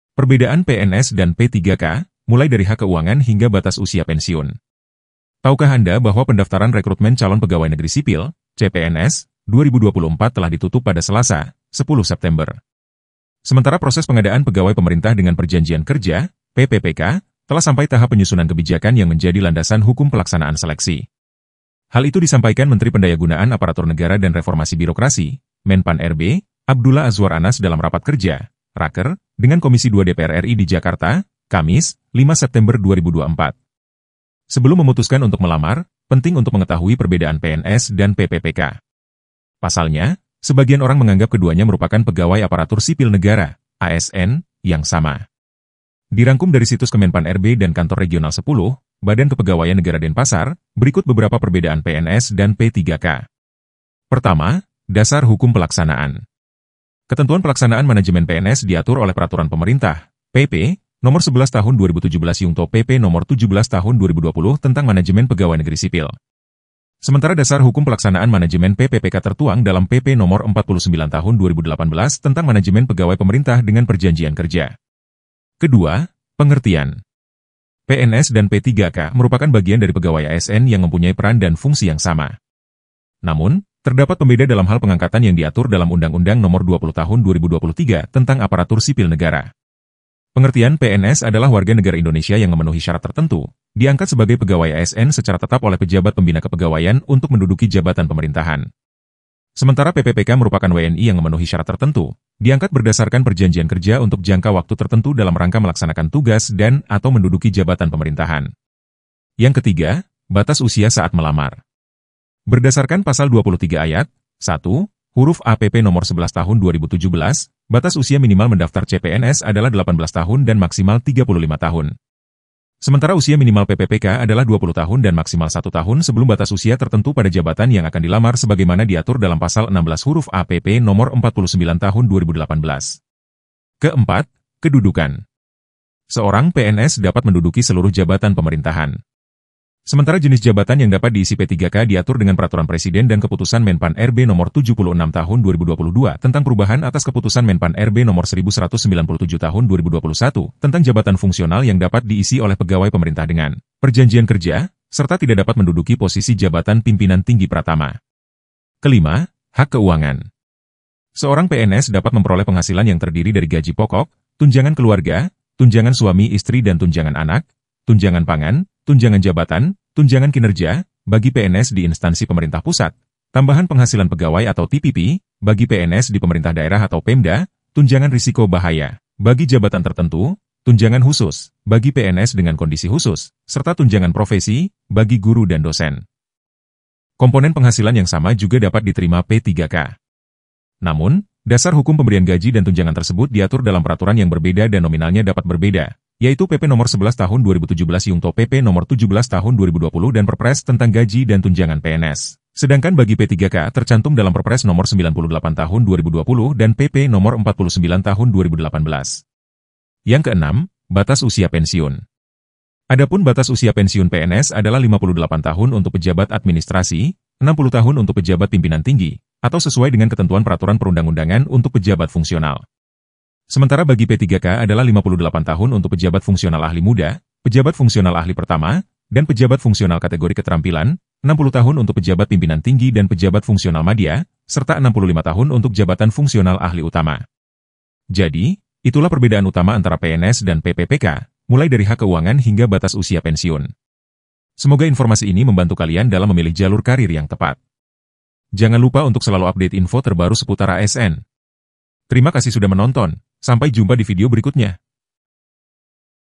PERBEDAAN PNS DAN P3K TERBARU! PRESIDEN TEKEN ATURAN TERBARU!
KENAIKAN GAJI BAGI PNS, PENSIUNAN, TNI, POLRI HARUS SEGERA DILAKSANAKAN.